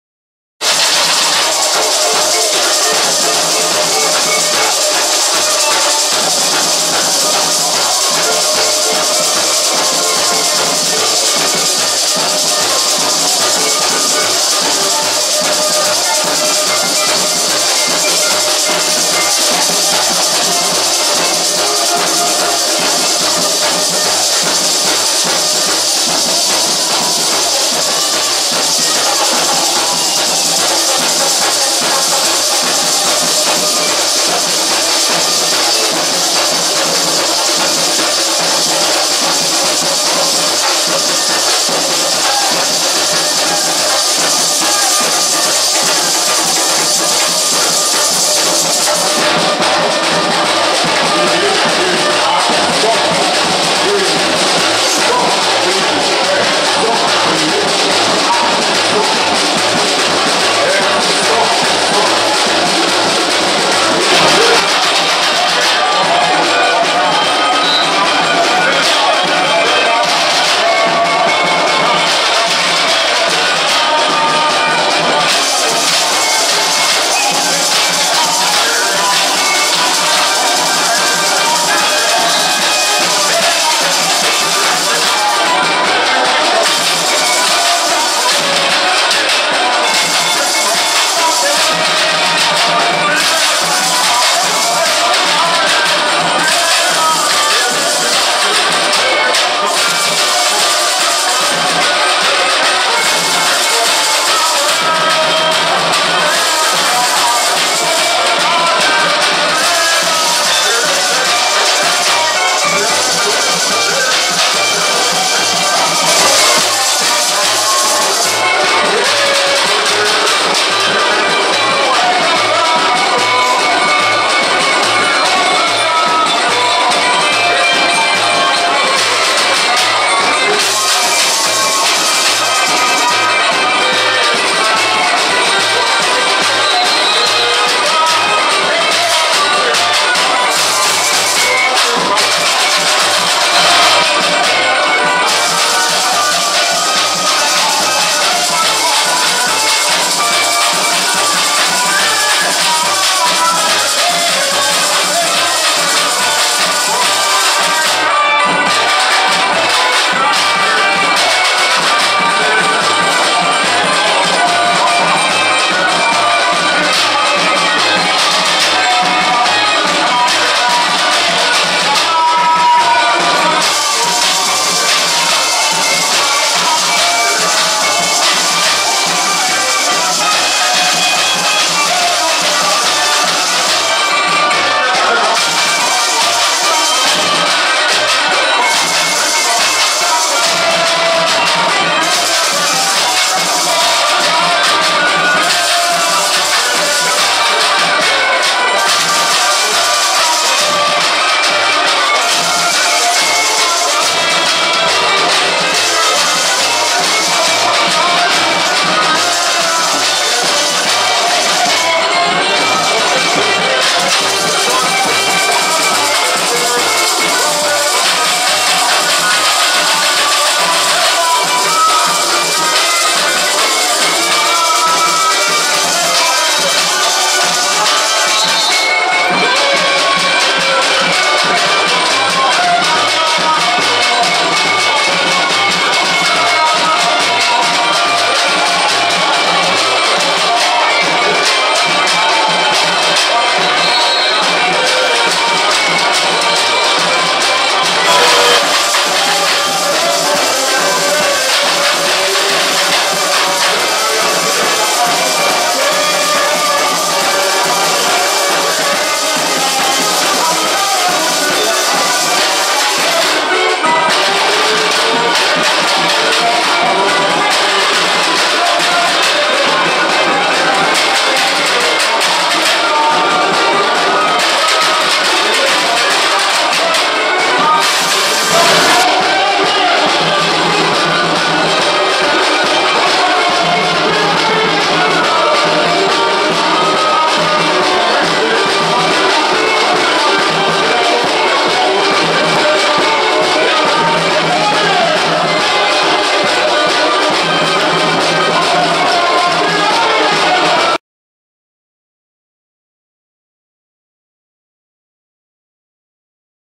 BOA VISTA NA FEIJOADA DA ANDARAÍ
Show da Bateria Águia Furiosa e Passistas da Independentes de Boa Vista, na feijoada da Nova Andaraí dia 28 de setembro de 2014 no Recreio dos Olhos.